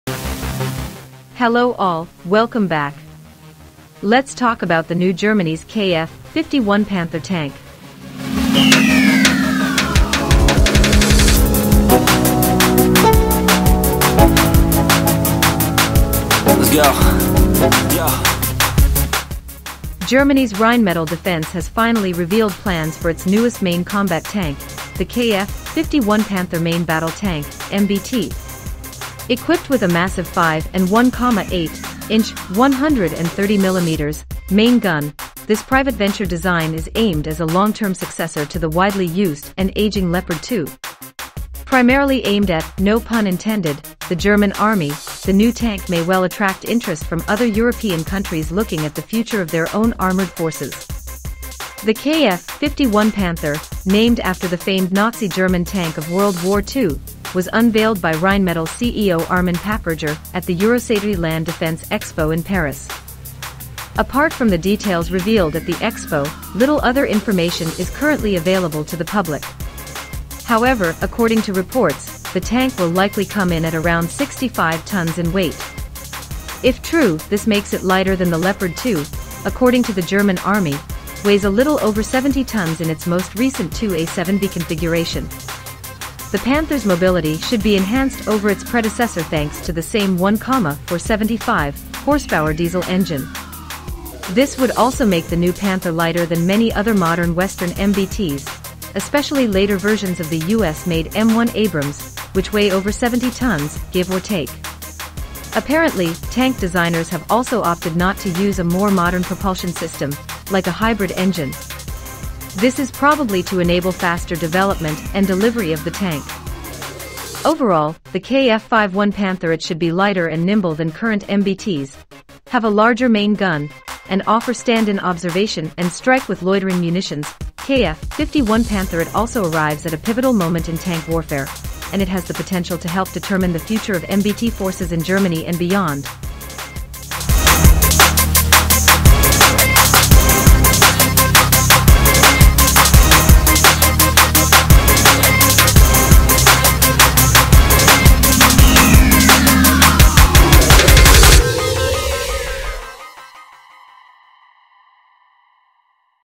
German Newest Fearsome KF-51 Panther, More Powerful Than Leopard 2 Tank
Germany Reveal The New Ultimate KF-51 Panther Main Battle Tank.

Hello all, welcome back. lets talk about the new germany's KF-51 Panther tank.Germany's Rheinmetall Defense has finally revealed plans for its newest main combat tank, the KF-51 Panther main battle tank (MBT). Equipped with a massive 5 and 1,8-inch (130 mm) main gun, this private-venture design is aimed as a long-term successor to the widely used and aging Leopard 2.

All the Images and Video is not ours, it's just an illustration.